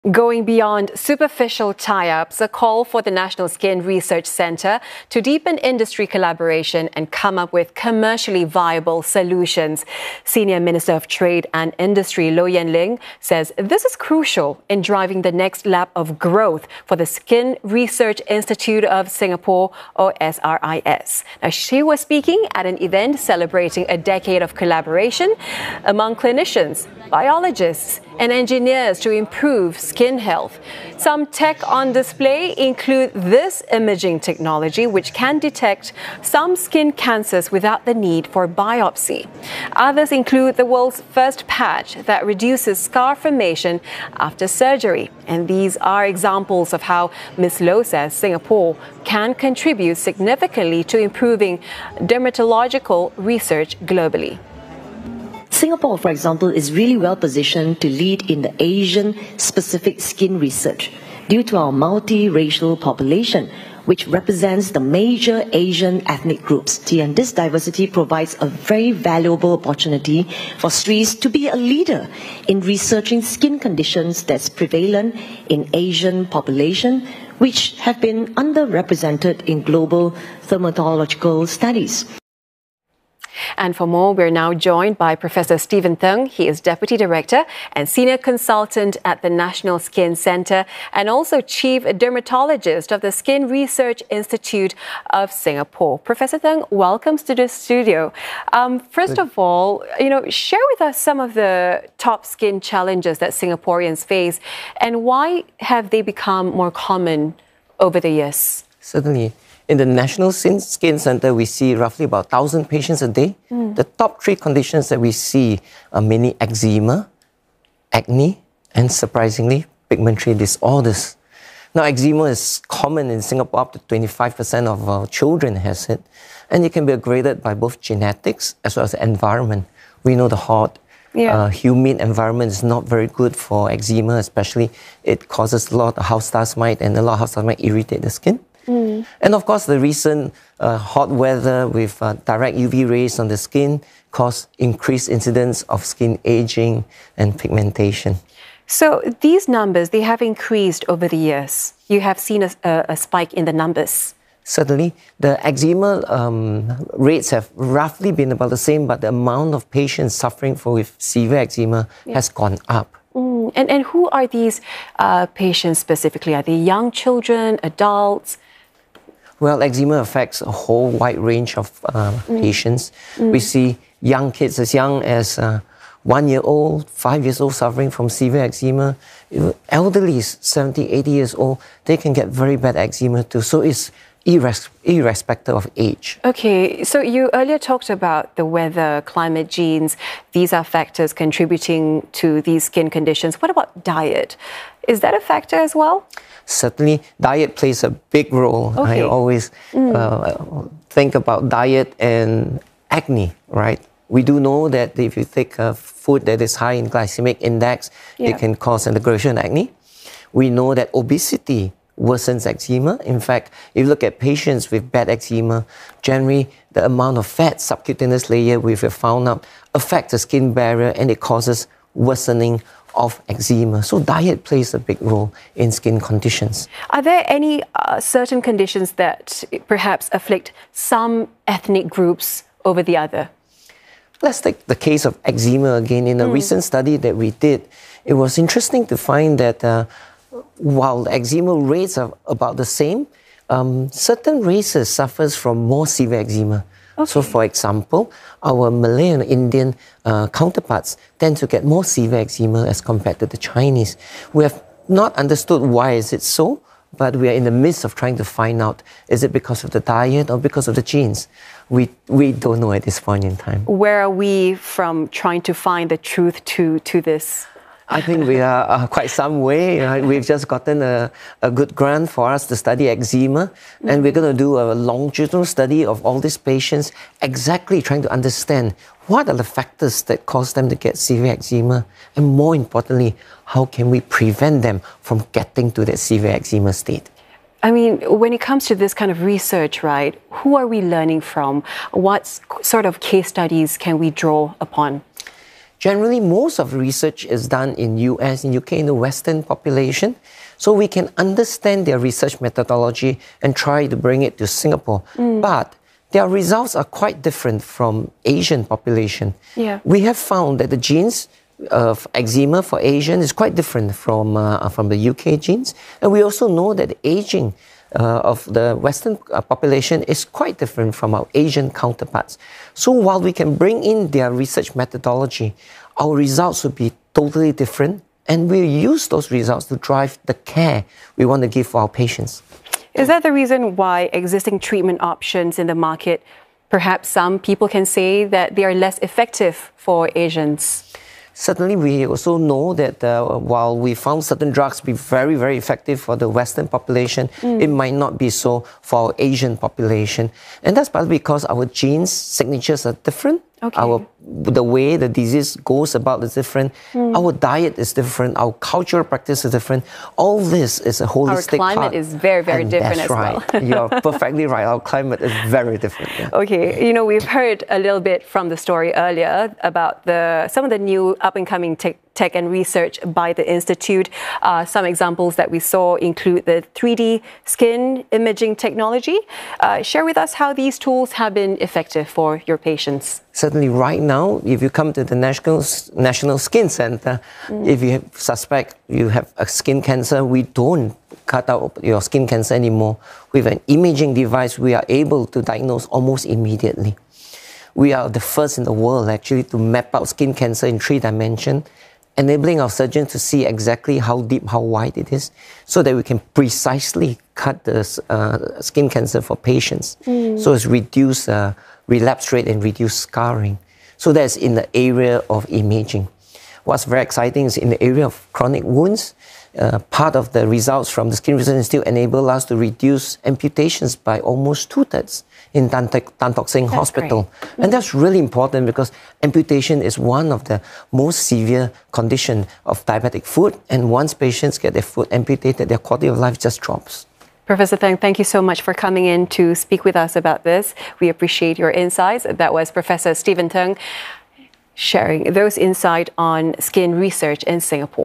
National Skin Centre's Steven Thng on top skin conditions that affect Singaporeans
Senior Minister of Trade and Industry Low Yen Ling has called for the Skin Research Institute of Singapore (SRIS) to deepen industry collaboration and come up with commercially-viable solutions. She said this is crucial in driving its next lap of growth. Professor Steven Thng, deputy director and senior consultant at the National Skin Centre and also chief dermatologist of the SRIS, shared more on the top skin conditions -- eczema, acne, pigmentary disorders -- ailing Singaporeans.